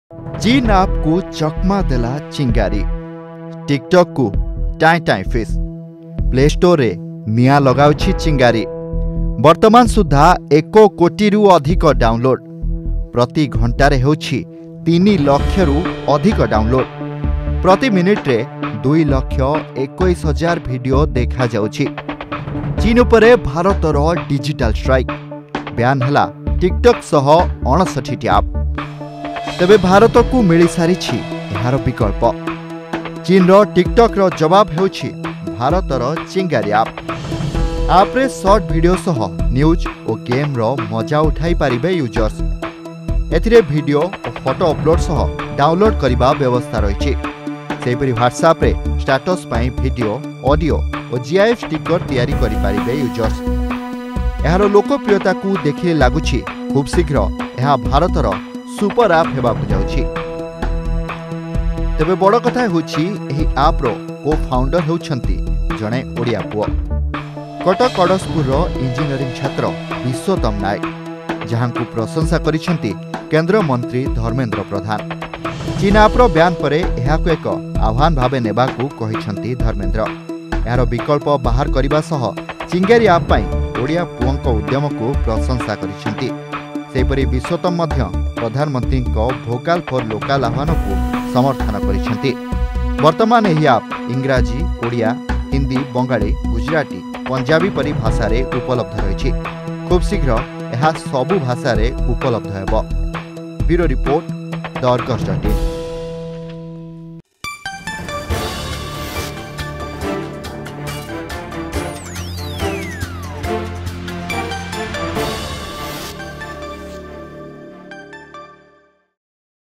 ଚୀନ୍ ଅ।ପର ଦେଶୀ ଜବାବ୍....
ଚୀନ୍ ଅ।ପର ଦେଶୀ ଜବାବ୍ । ଟିକଟକର ଲୋକପ୍ରିୟତାକୁ ଟକ୍କର ଦେଉଛି, ଭାରତର ଚିଙ୍ଗାରୀ ଅ।ପ୍ । ଯାହାକୁ 1 କୋଟିରୁ ଅଧିକ ୟୁଜର୍ସ ଡାଉନଲୋଡ କରିସାରିଛନ୍ତି.